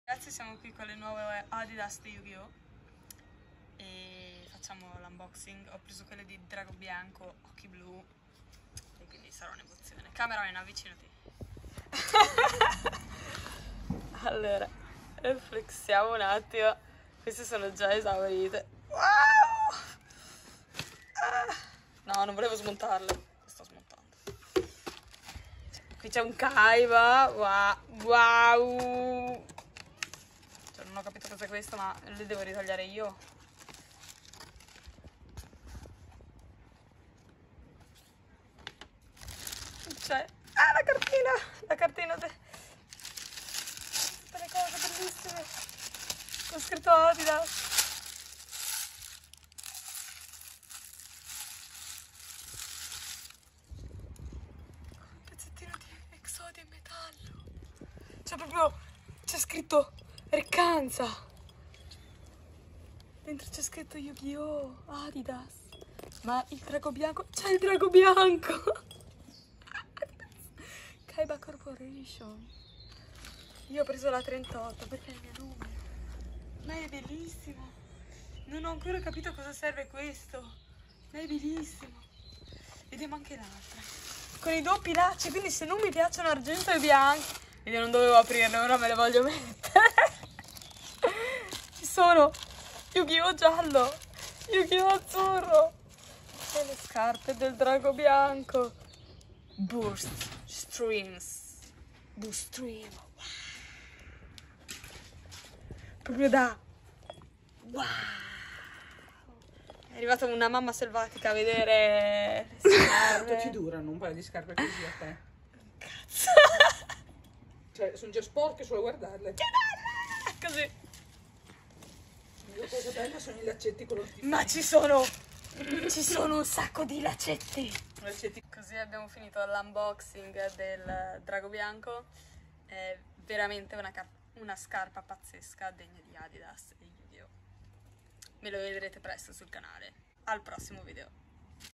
ADIDAS X YUGIOH UNBOXING! CHE PULL!
Prometto che il cameramen in questo video non è stato maltrattato 😂 (grazie sister per il Video ❤️)
Ecco a Voi L’unboxing delle nuove Scarpe Adidas X Yugioh.